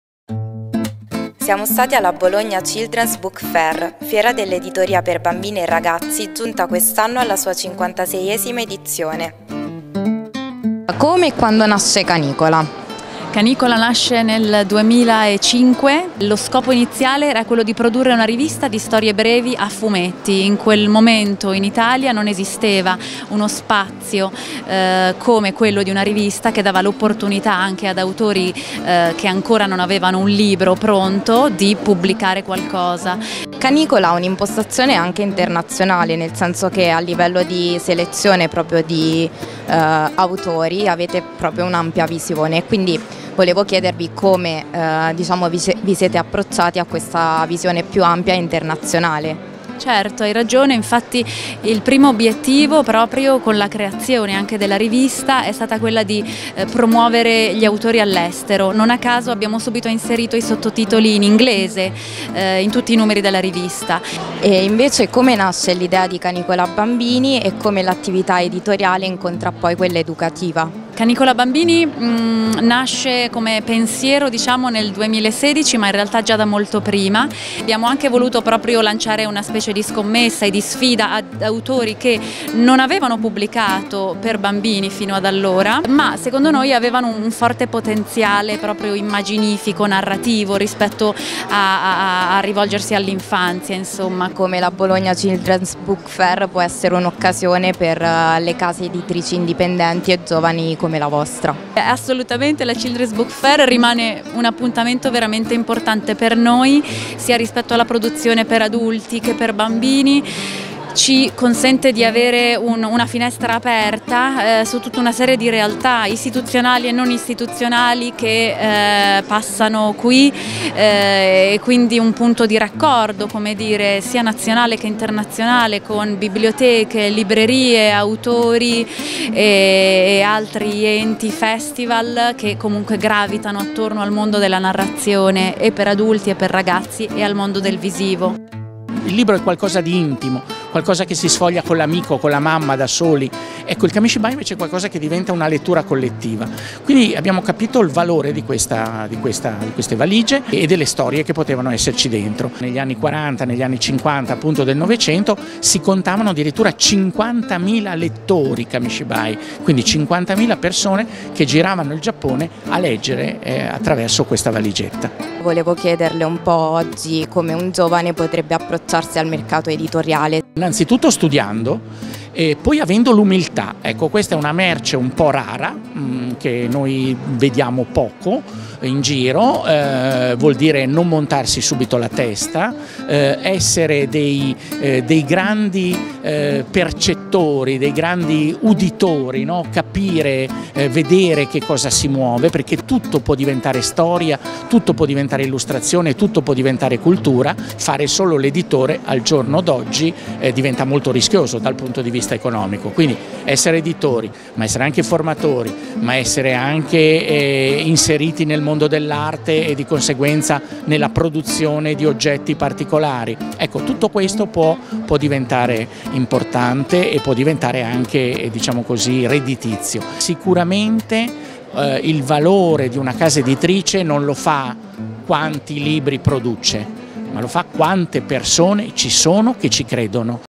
Childrens' Book Fair Bologna 2019
Grande entusiasmo e partecipazione alla 56° edizione della Childrens' Book Fair di Bologna!